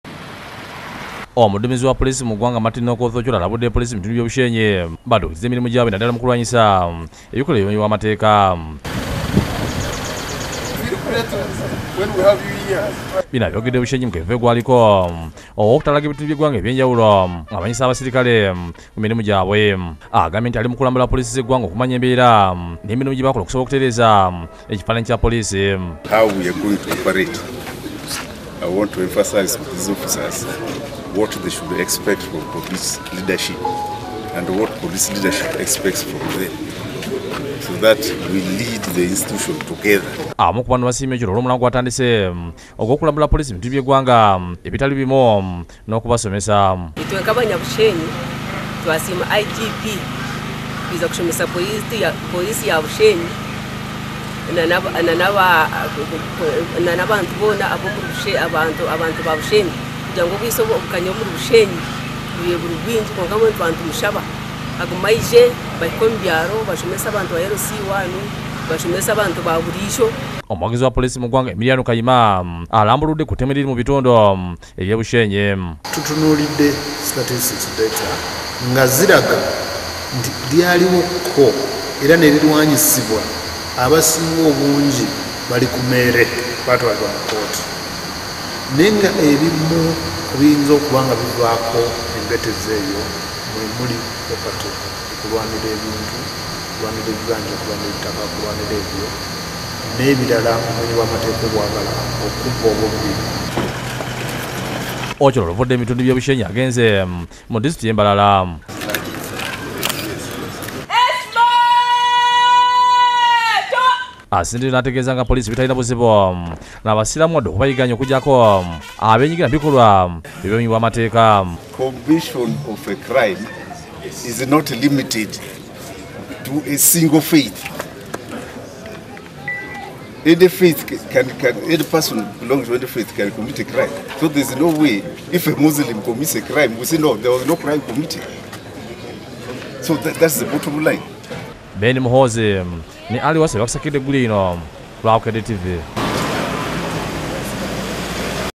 IGP Ochola alambudde Poliisi ez'enjawulo.
Akubirizza abaserikale okukolera awamu.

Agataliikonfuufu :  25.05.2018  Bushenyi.
Reporter:  Ali Wasswa ne Ben Muhoozi.